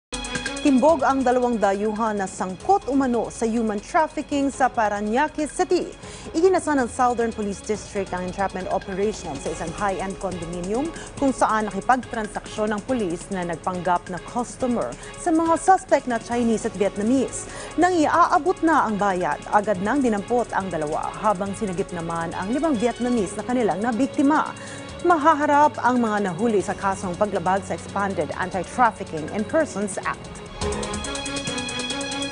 2 dayuhan, arestado sa Parañaque City dahil sa pambubugaw umano sa 5 Vietnamese
2 dayuhan, arestado sa Parañaque City dahil sa pambubugaw umano sa 5 Vietnamese